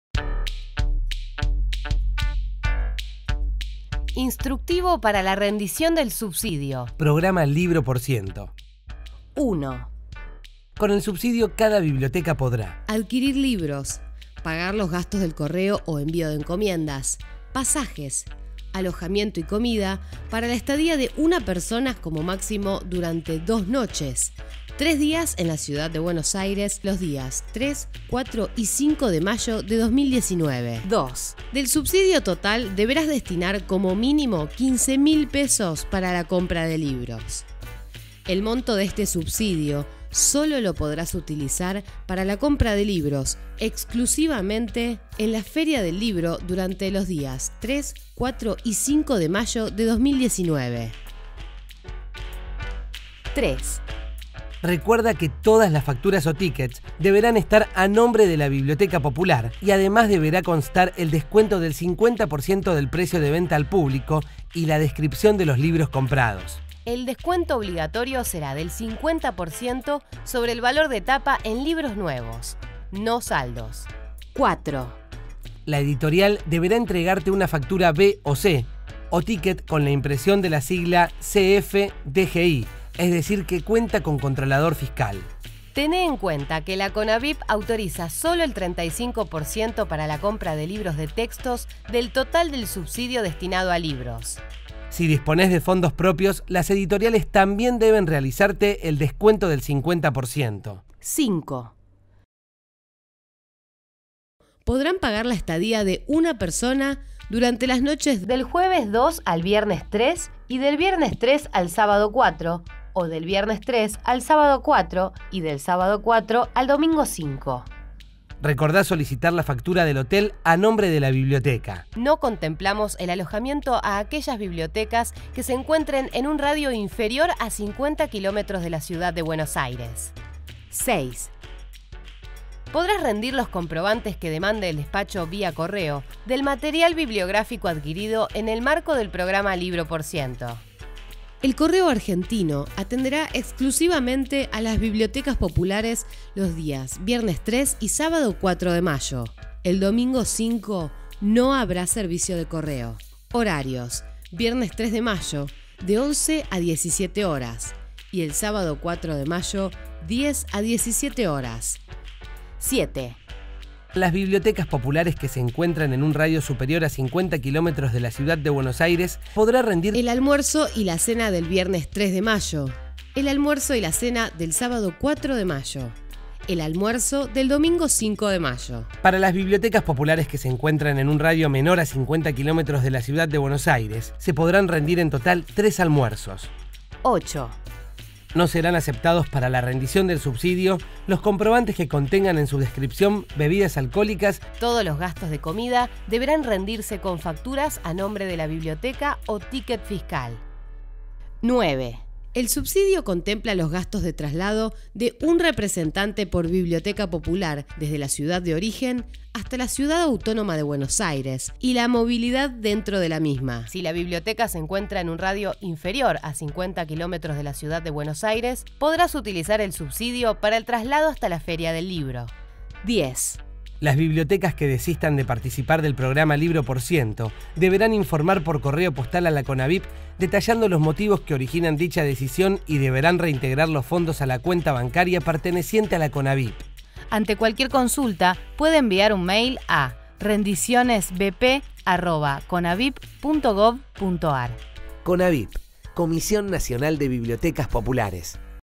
Video Tutorial Rendición de subsidio Libro % 2019
Video tutorial con la información para la rendición del subsidio del Programa Libro % 2019 que lleva adelante la CONABIP en la 45 Feria del libro de Buenos Aires.